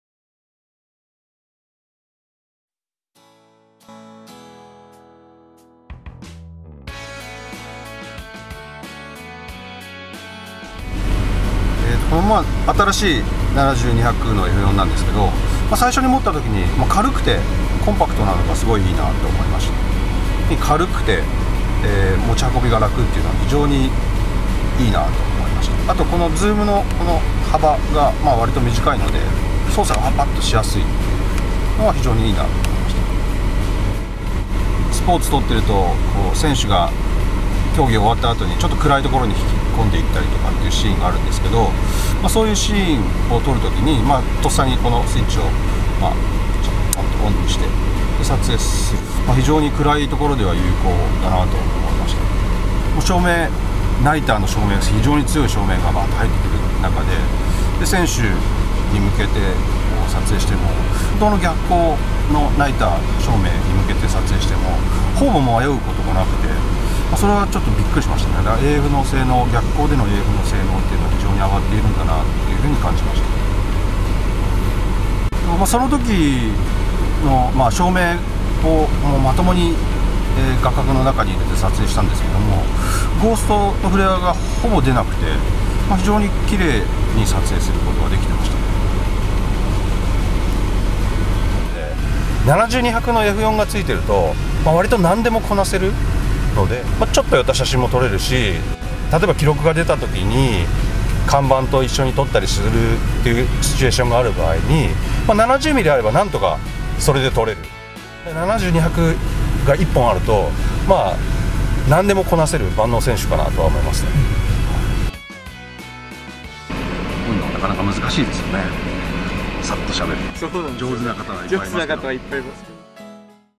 キヤノン EF70-200mm F4L IS II USM 報道カメラマンはこう使う！
望遠ズームレンズ「キヤノン EF70-200mm F4L IS II USM」の気になる性能は？第102回日本陸上競技選手権大会の撮影で使用した報道カメラマン・奥井隆史さんの実写インプレッション。